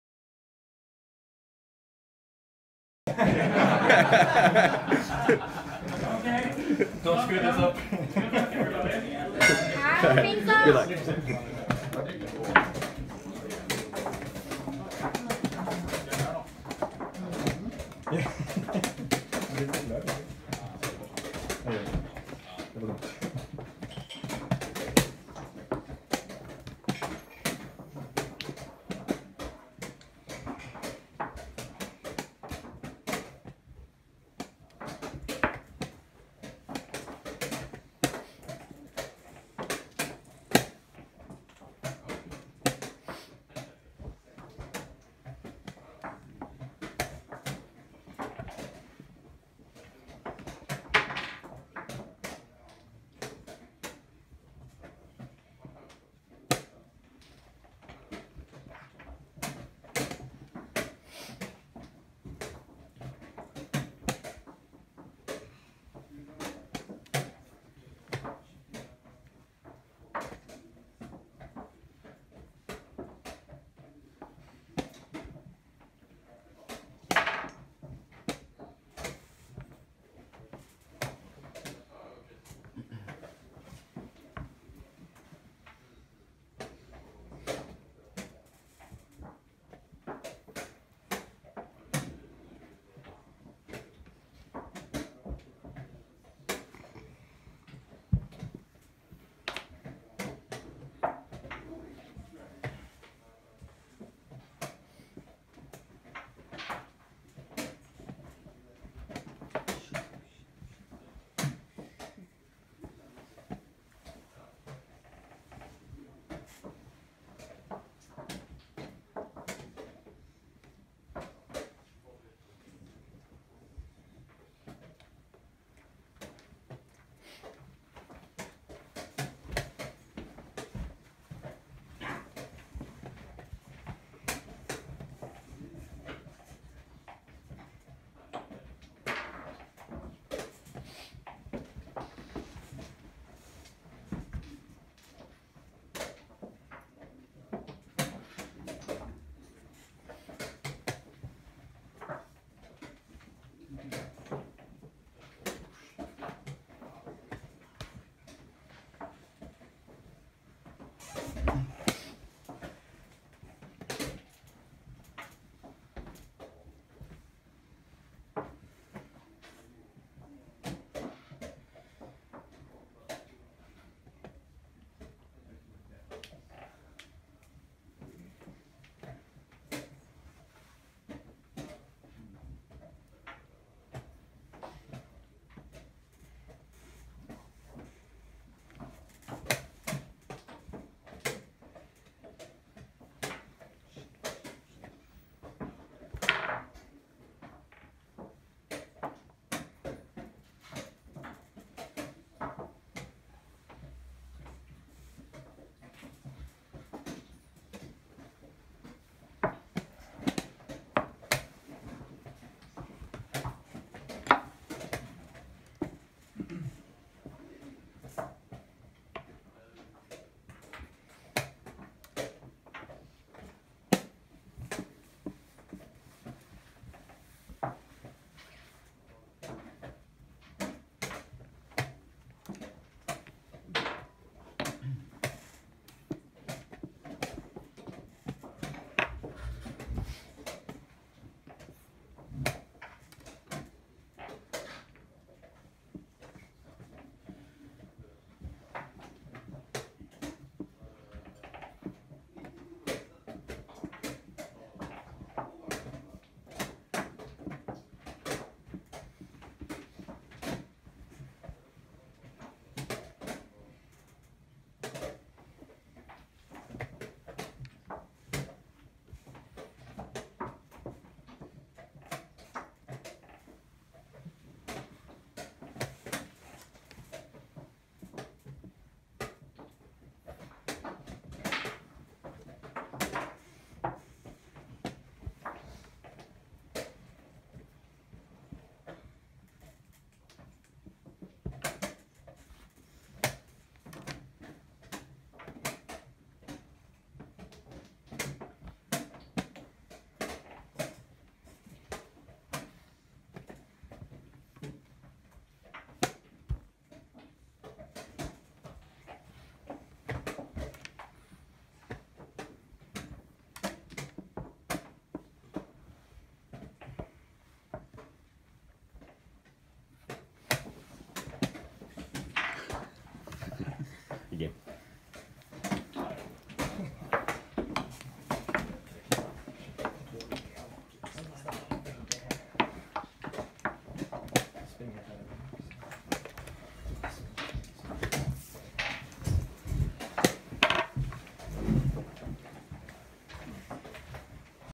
Stjernen Blitz: Rueda-Kjedlsen vs. IM Bartholomew
A blitz game from the meetup on Oct. 8, 2017 at Stjernen Sjakklubb in Oslo, Norway. We played a 7-round tournament at a time control of 3 minutes with a 2 second increment. This is from Round 7. Thanks to Oddbjørn Sjøgren for the footage!

White: Juan Carlos Rueda-Kjeldsen (FIDE: 1988)
Black: IM John Bartholomew (FIDE: 2444)